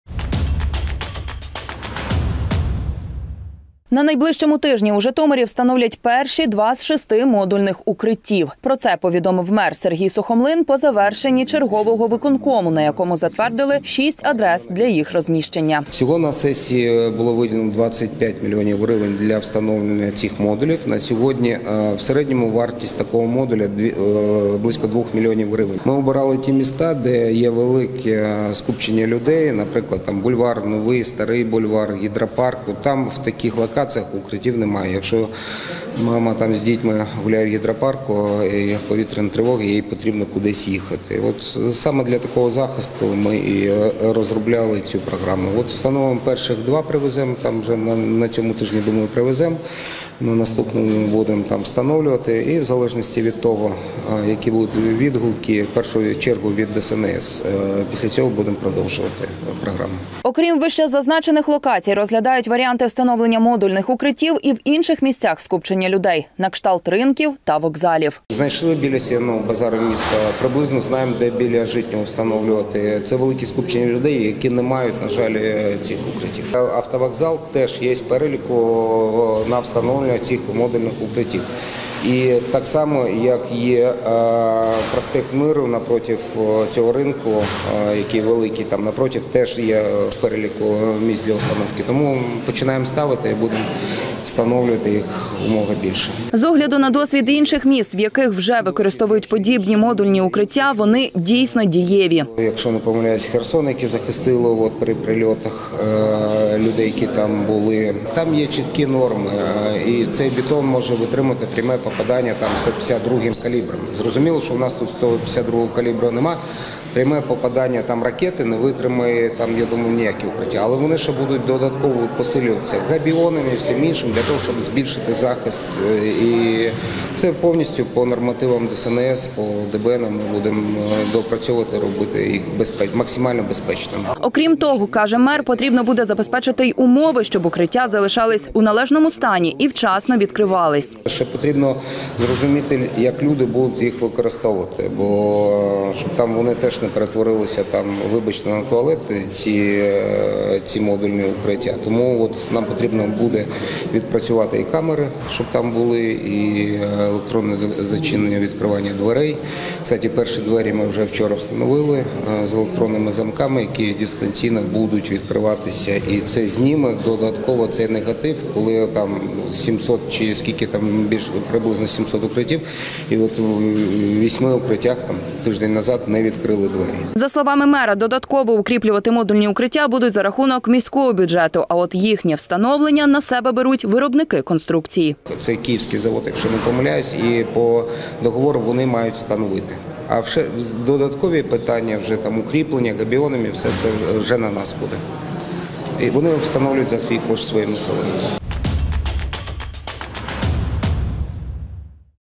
Мер Житомира про встановлення модульних укриттів у місті
На найближчому тижні у Житомирі встановлять перші два з шести модульних укриттів. Про це повідомив мер Сергій Сухомлин по завершенні чергового виконкому, на якому затвердили 6 адрес для їх розміщення. А саме: Новий бульвар, 4, площа Польова, 10, Лятошинського, 13, Покровська, 131, Старий бульвар, 14а, Чуднівська, 167.